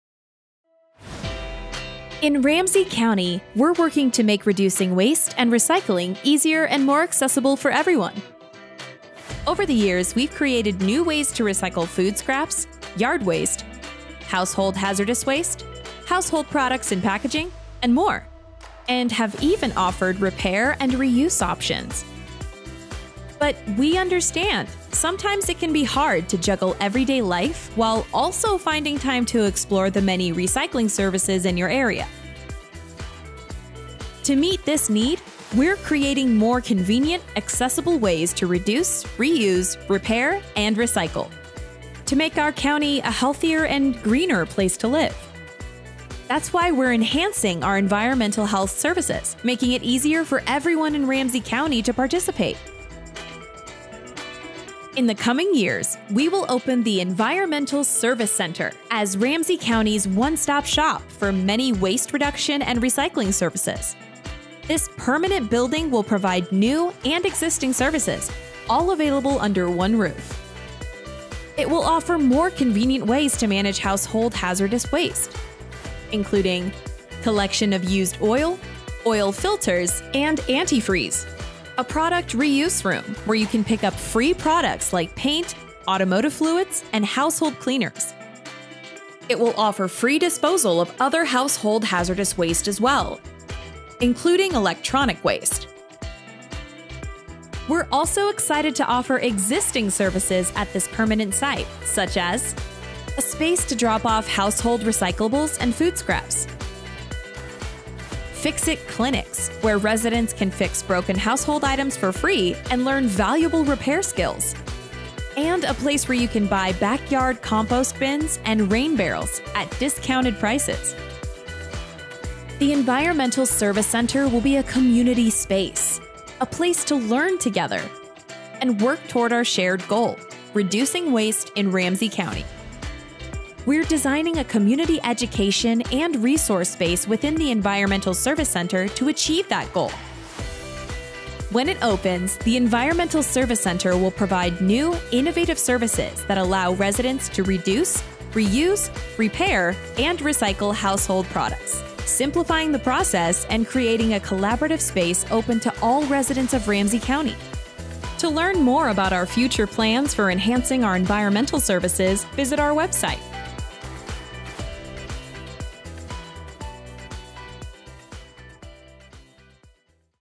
Environmental Service Center Overview
The county is building a new Environmental Service Center to make it easier for residents to reduce waste, reuse and recycle.